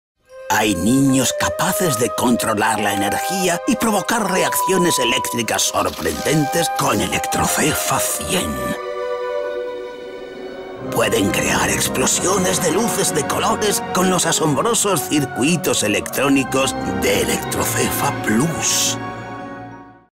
Juego Electrocefa 100 circuitos Cefa
Construye más de 100 circuitos y proyectos de electricidad gracias a Electrocefa 100. El juego contiene más de 30 elementos entre los que se incluyen circuitos, interruptores, placa base y un completo manual de instalación y experiencias a realizar.